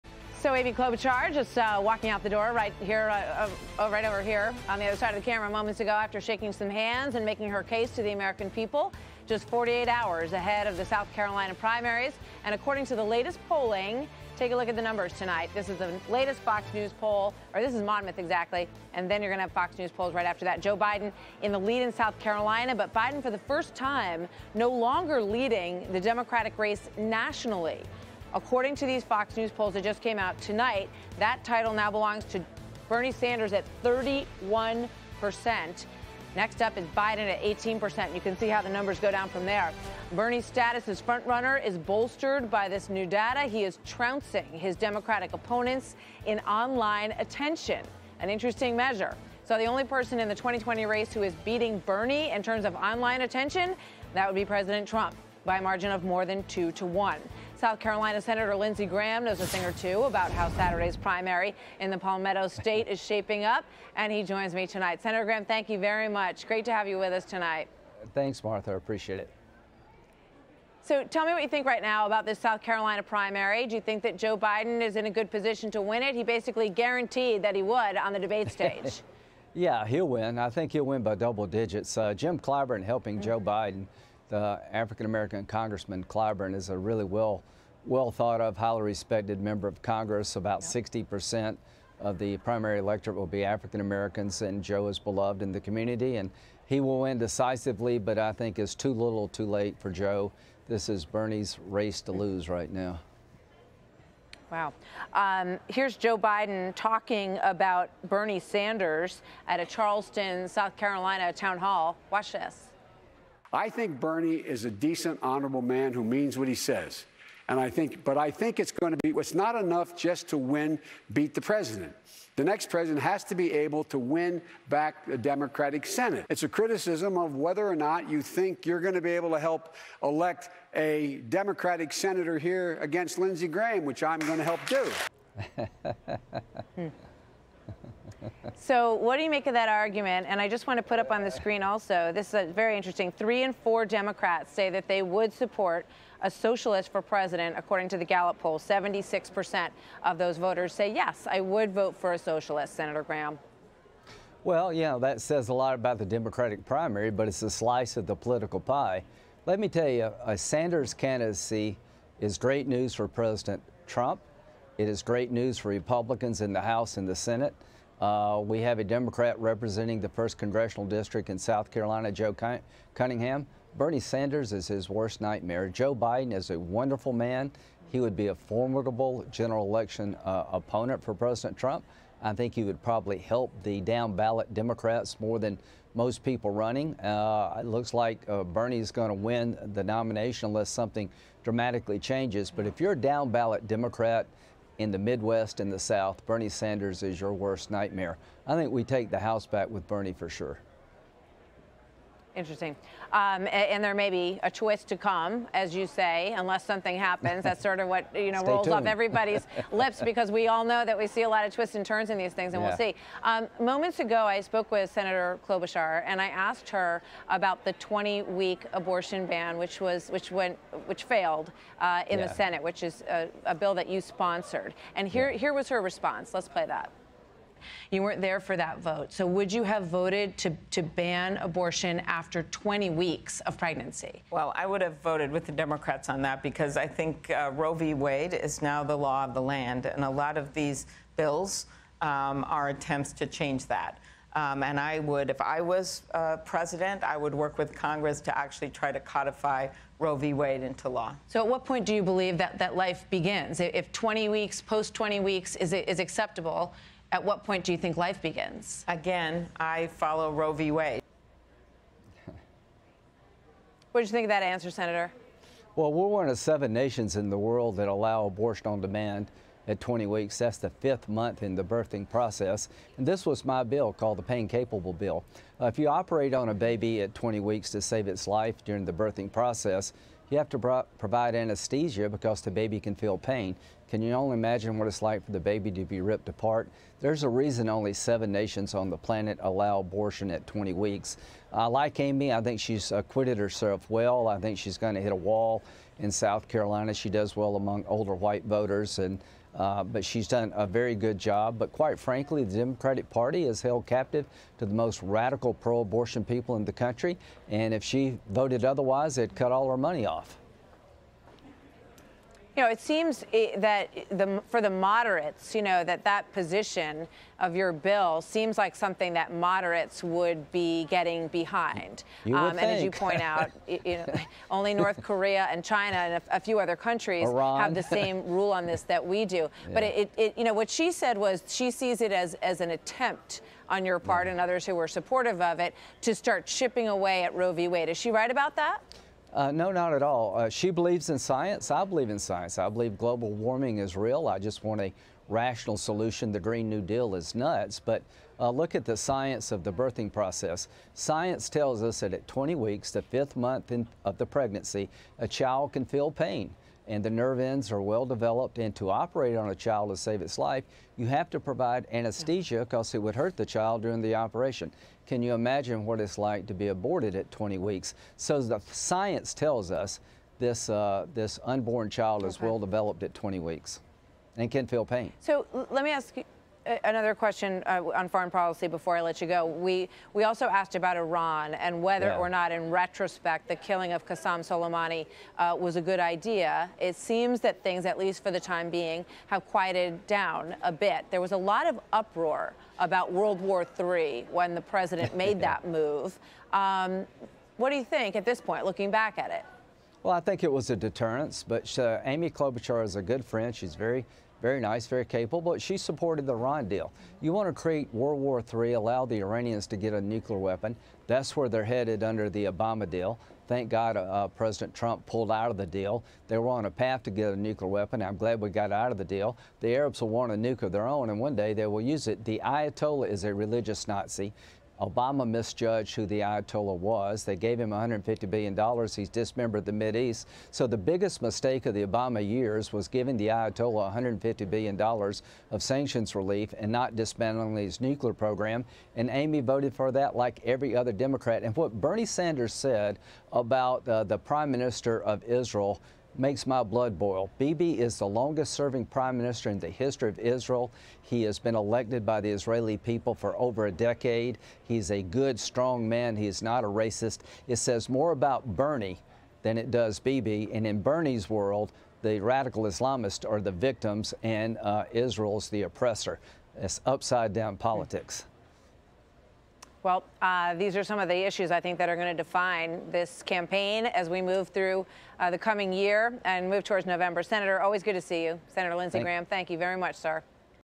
Graham: Sanders’ candidacy is a down-ballot Democrat's worst nightmare
Senator Lindsey Graham, R-SC, shares predictions for his state's primary, criticizes Bernie Sanders' policies.